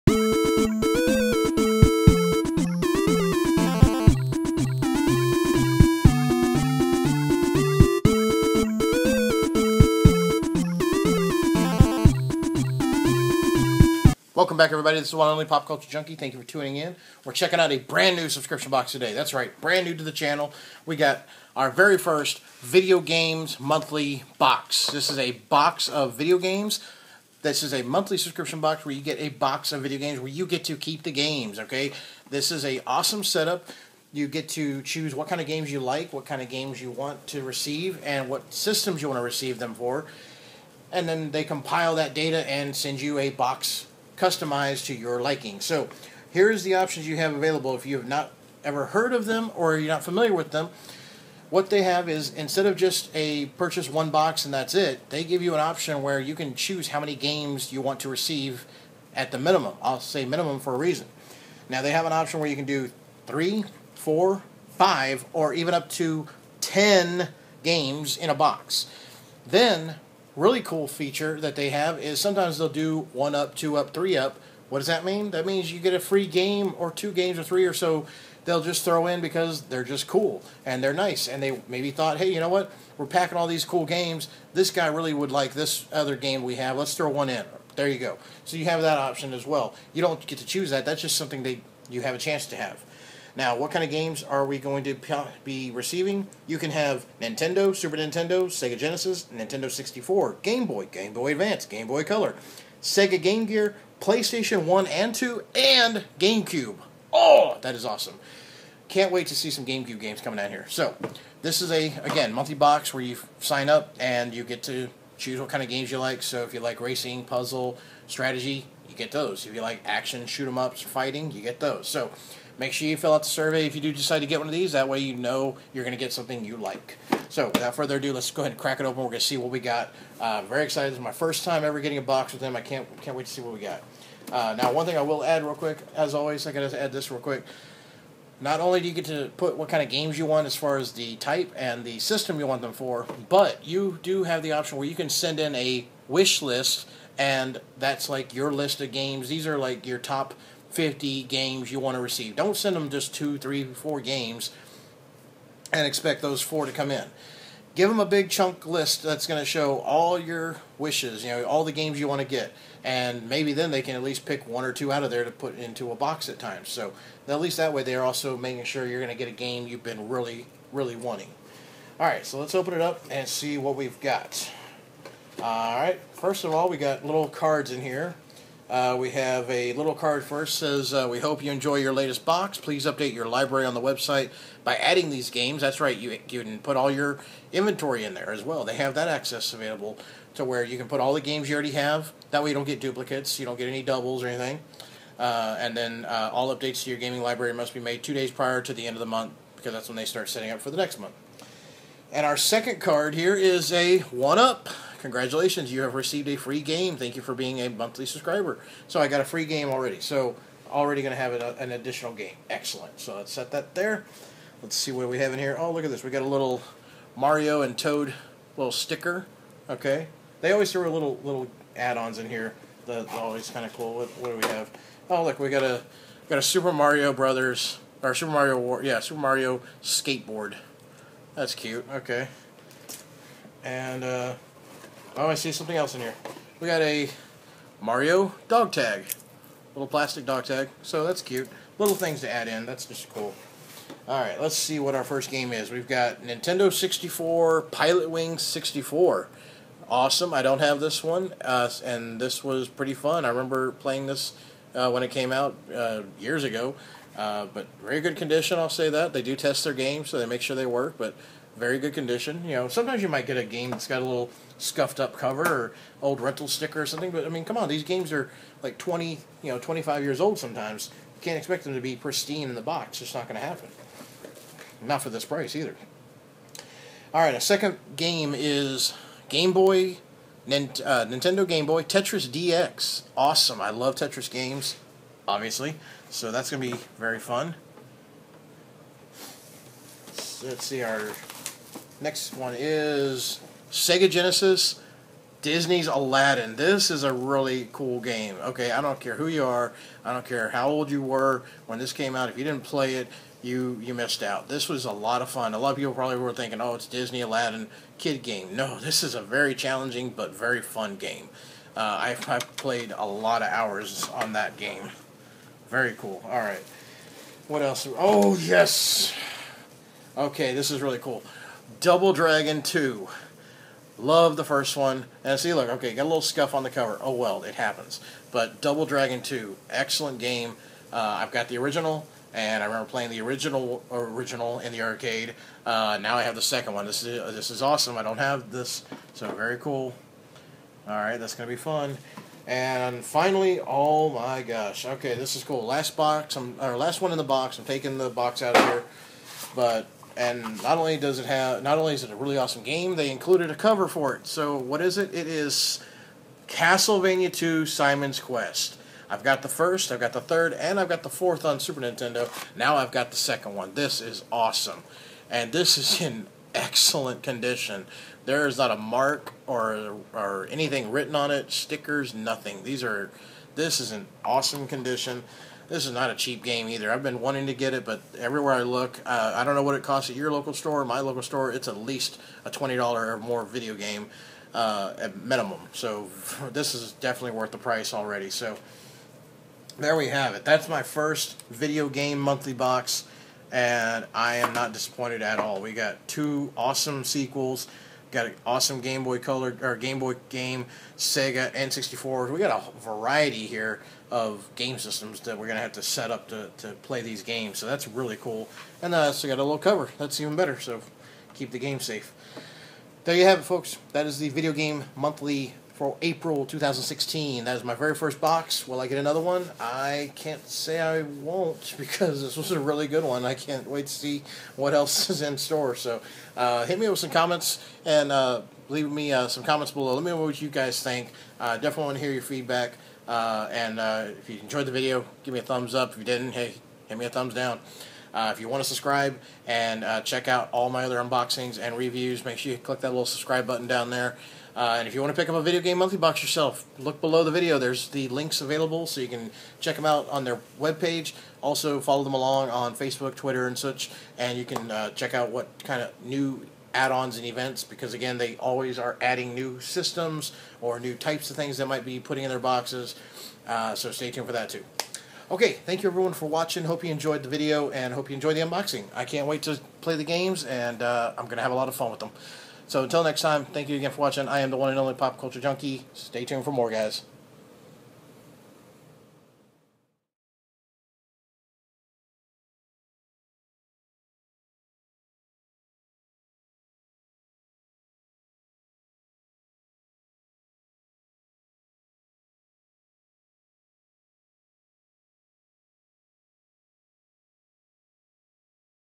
Video Games Monthly Unboxing & Review April 2016 Subscription Box
Pop-Culture Junkie unboxes the very first VIDEO GAMES MONTHLY Subscription box.